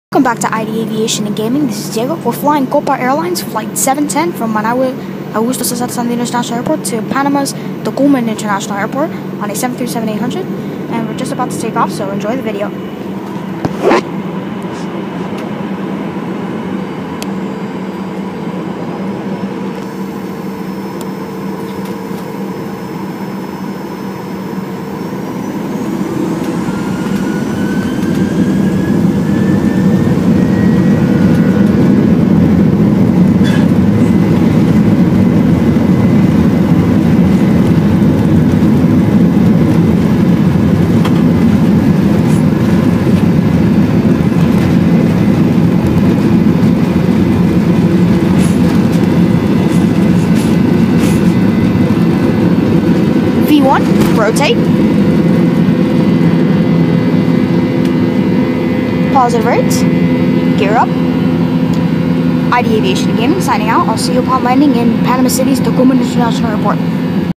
ENGINE VIEW takeoff from Managua Augusto César Sandino Airport (MGA)
Diego took a Copa 737-800 to Panama City, and this time he took an amazing engine view.